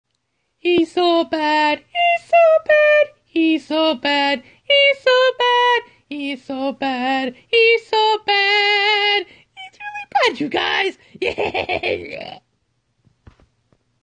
bad singing
silly doggy head literally singing about being bad.

Why is the video a circle? Recorded on Telegram